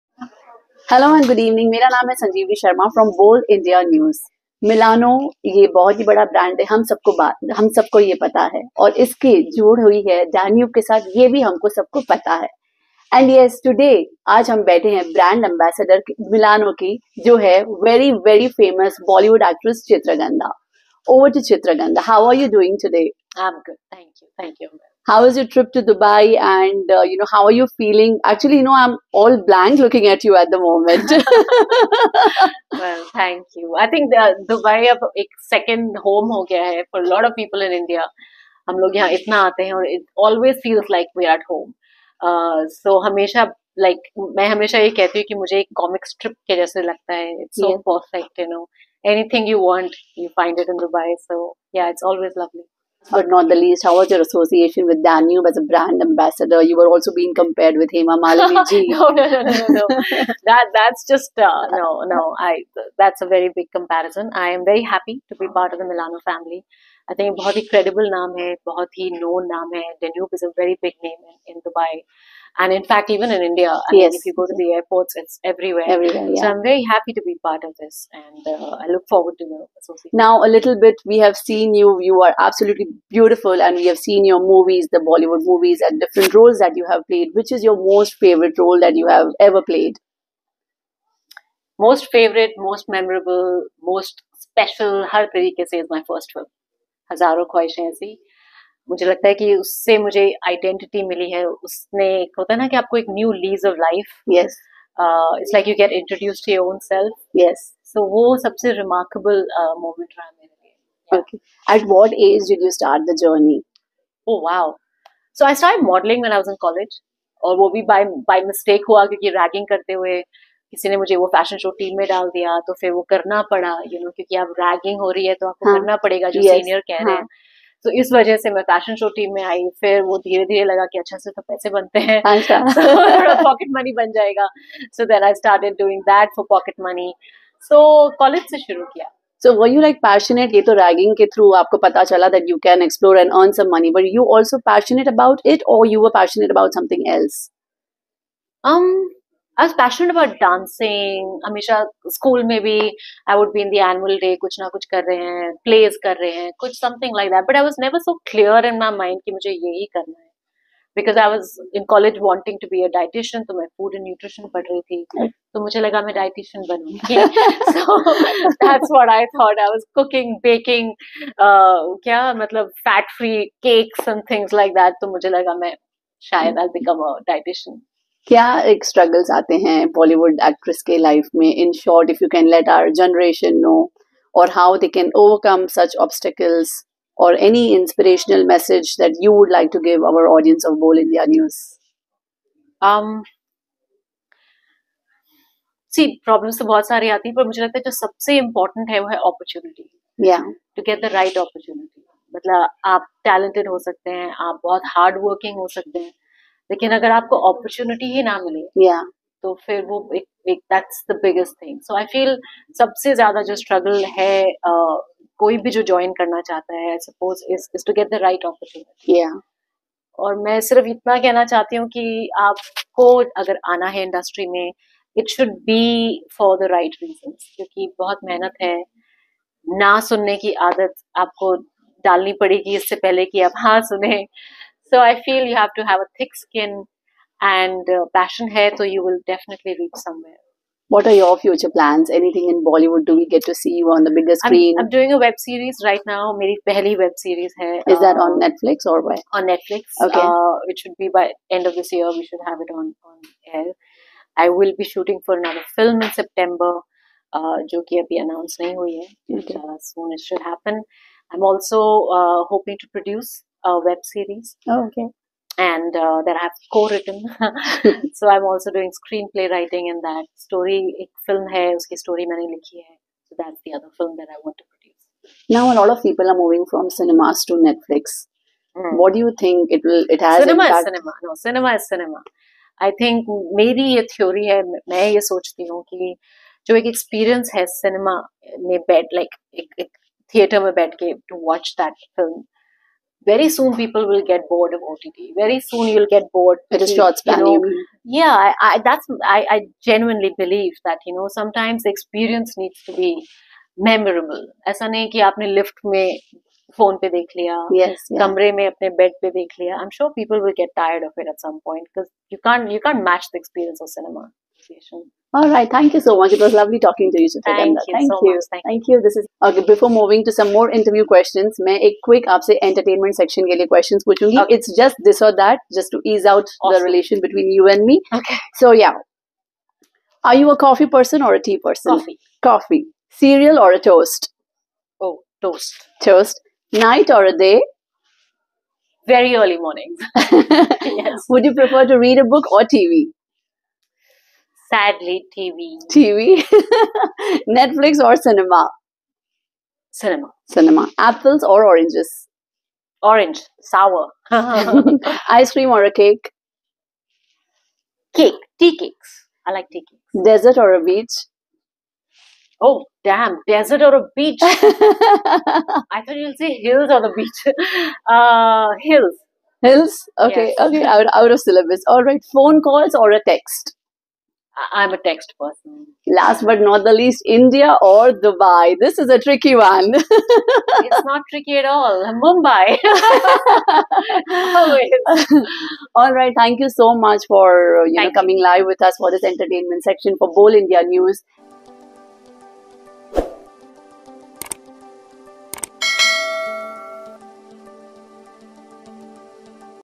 Milano Brand Ambassador Chitrangada Singh Exclusive interview with bol india news Sanjivani Sharma
Milano Brand Ambassador Chitrangada Singh Exclusive interview with Sanjivani Sharma bol india news